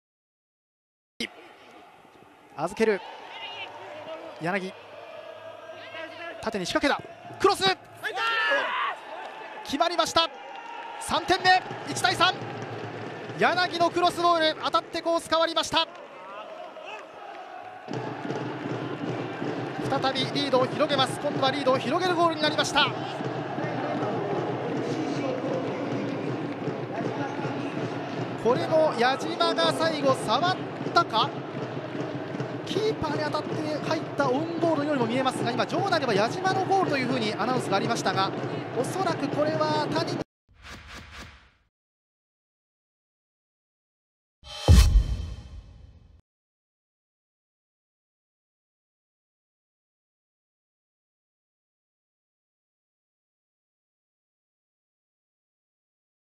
【公式】ゴール動画：柳 貴博（Ｆ東23）54分 ガンバ大阪Ｕ－２３vsＦＣ東京Ｕ－２３ 明治安田生命Ｊ３リーグ 第8節 2018/5/26
PA内で深い位置まで侵入した柳 貴博（Ｆ東23）のクロスが相手選手に当たってゴールに吸い込まれる！明治安田Ｊ３のゴール動画を公開中！
2018年5月26日（土）に行われた明治安田生命Ｊ３リーグ 第8節 Ｇ大23vsＦ東23のゴール動画です。